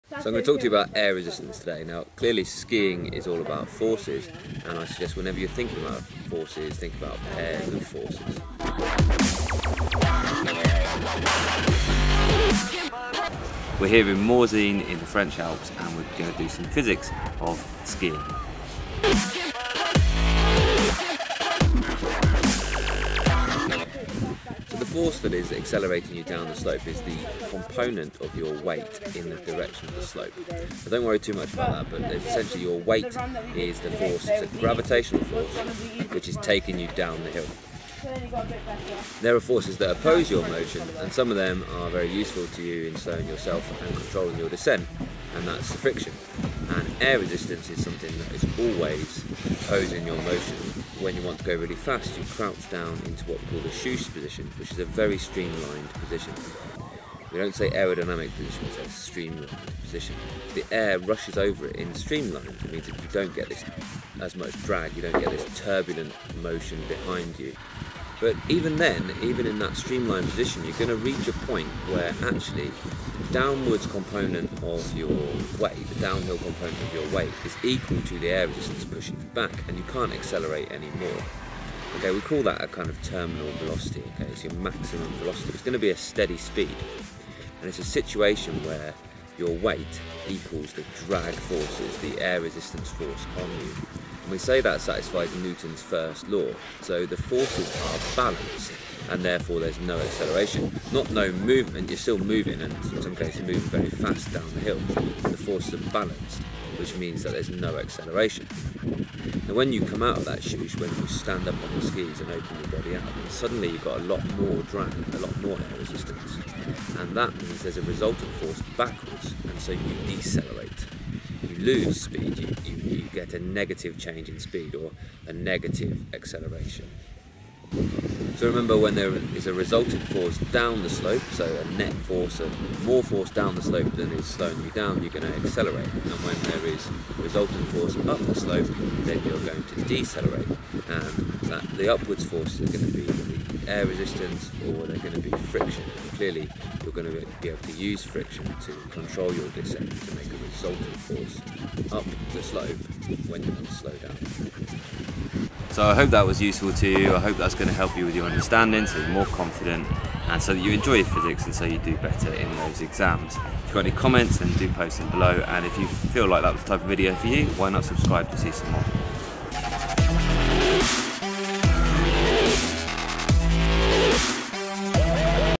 Air Resistance - Skiing Physics - GCSE and A Level Physics
A good application of Newton's First and Second Law.  When there is a resultant force down the slope you'll accelerate, when the forces are balanced you travel at a constant speed, and when there is a resultant force up the slope you'll decelerate.

Skiing is all about forces, find out about the forces involved when you are trying to go as fast as you possibly can!

It's also really useful knowledge and understanding for you GCSE or A Level Physics!